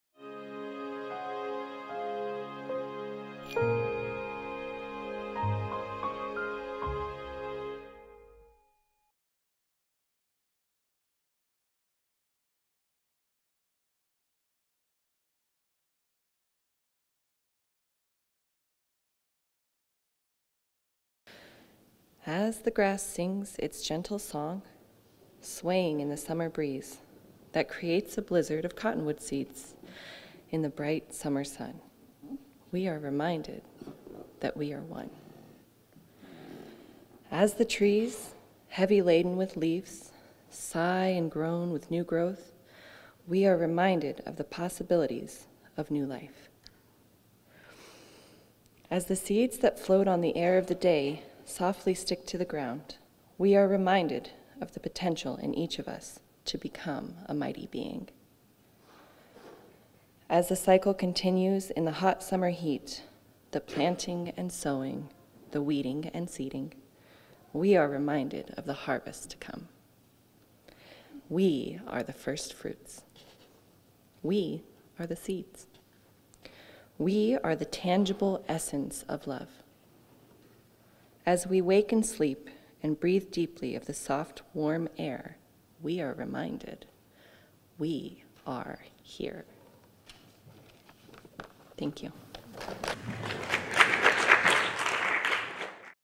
Courtney Herrera Reads an Original Poem
June 10, 2018: Sunrise Ranch resident Courtney Herrera reads an original poem called "As the Grass Sings" during the Sunday service in the Dome at Sunrise Ranch, Loveland, Colorado.

Visit: SunriseRanch.org